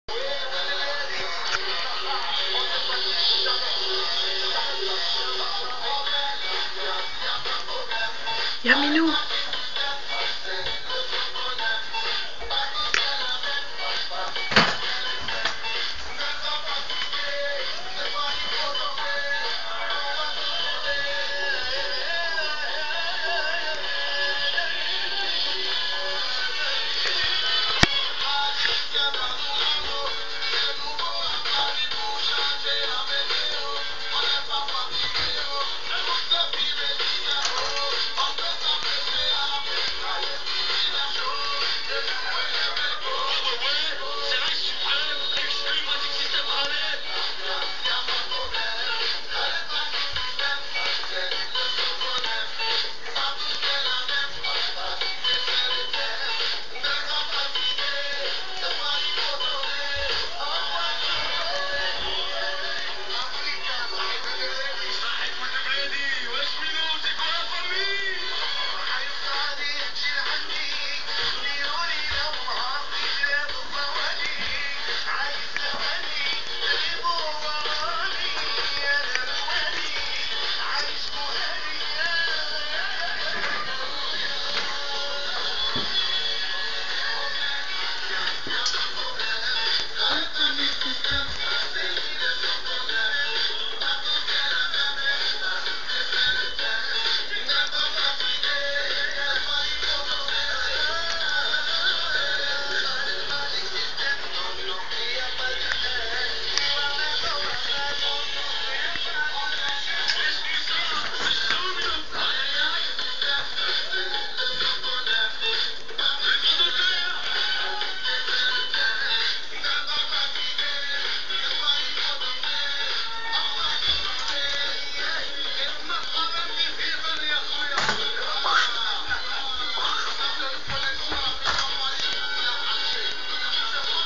yohan benabdi
Voilà yohan qui danse ou qui essaiye de dansé du RAP !
SVP PAS DE COMMENTAIRE DEGUELLASE!!
MERCII